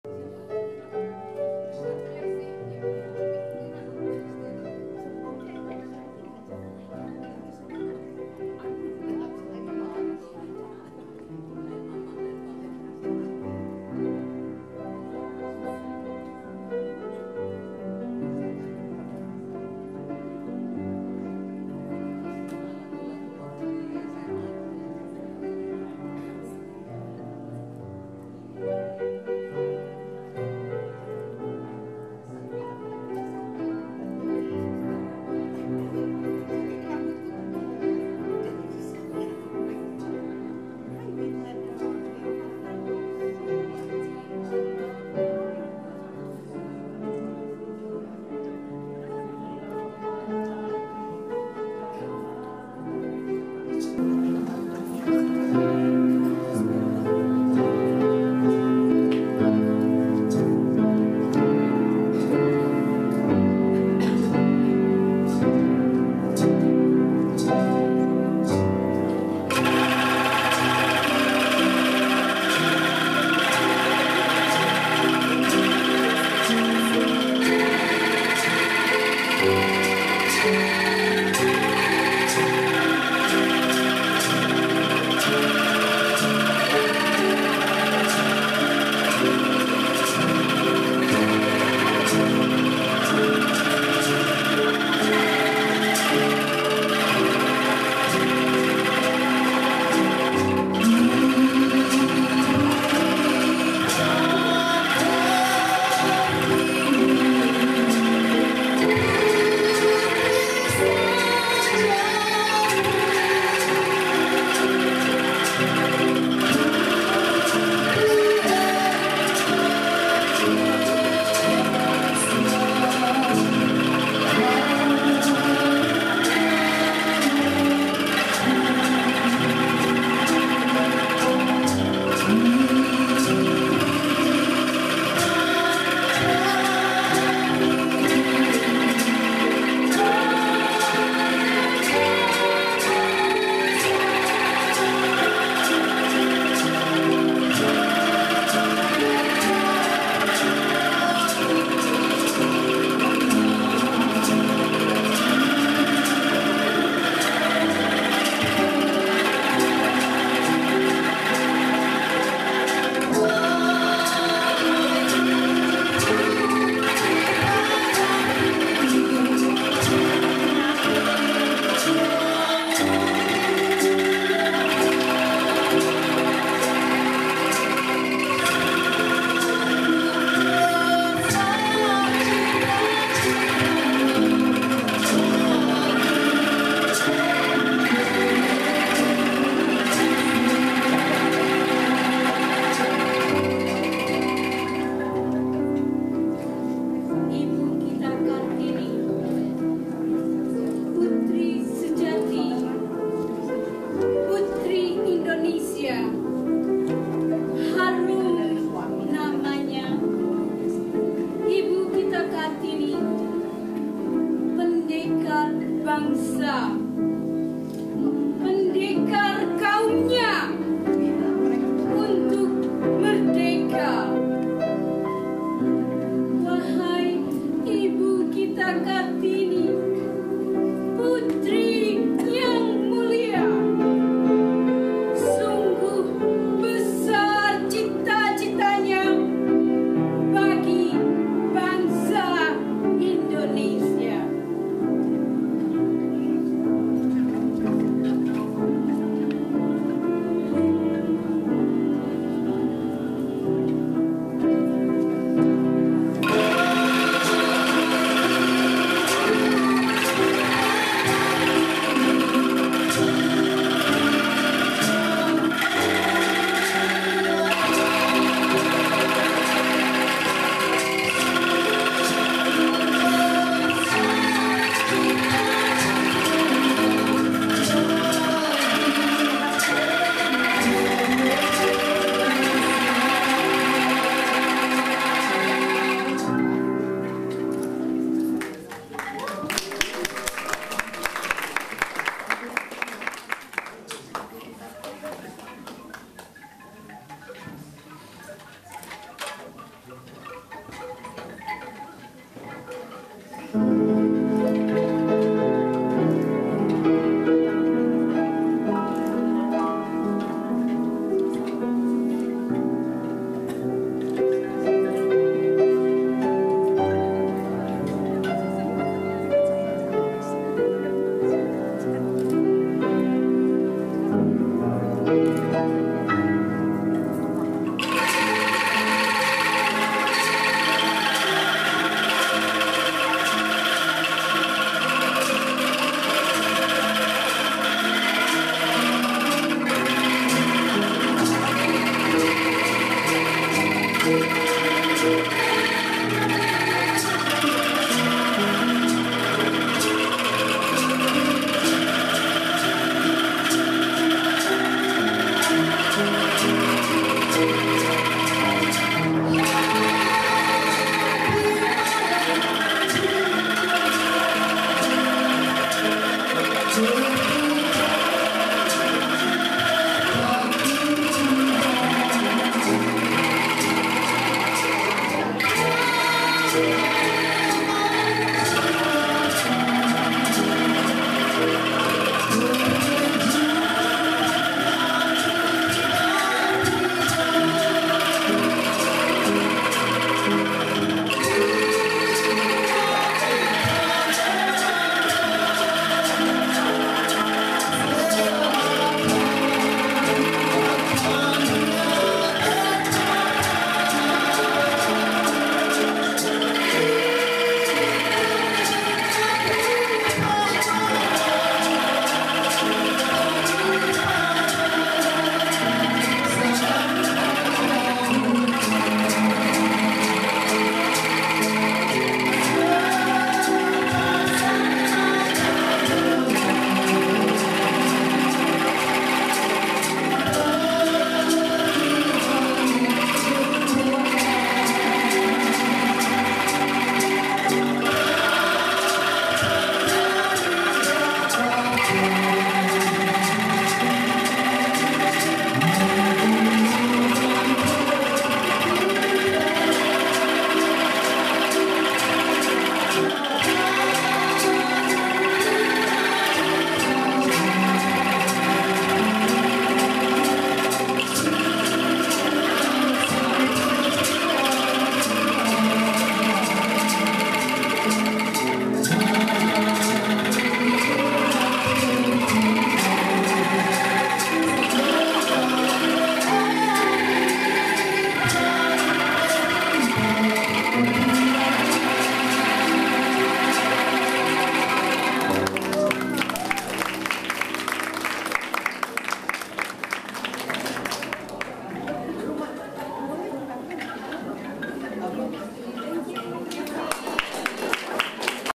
Angklung Cendrawasih Hari kartini 2015 (HD)
Memperingati Hari Kartini 25 April 2015 di Wisma Indonesia - San Francisco CA.